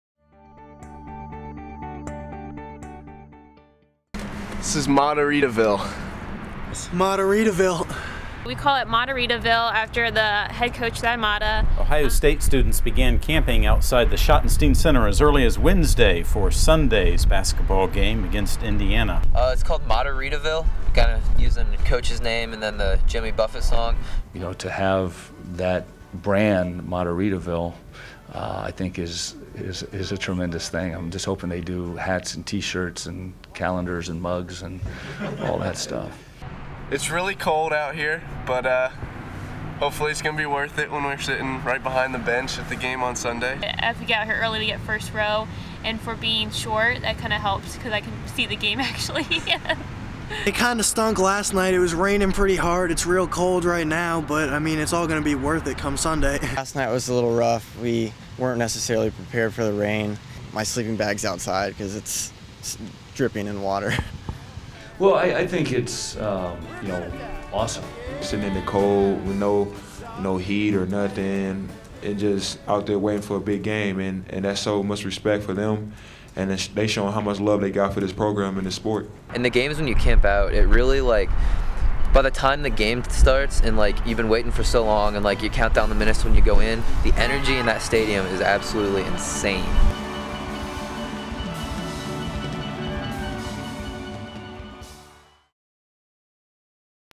Mattaritaville: Ohio State basketball fans camp outside Schott
Ohio State basketball fans camp five full days before the start of the Ohio State Basketball game against Indiana. Campers included Evan Moore, Jake Johnson, Joey Rich, and Tyler Duffy.  Coach Thad Matta and player Deshaun Thomas weigh in with their appreciation. Columbus Dispatch video by Doral Chenoweth recorded February 8, 2013.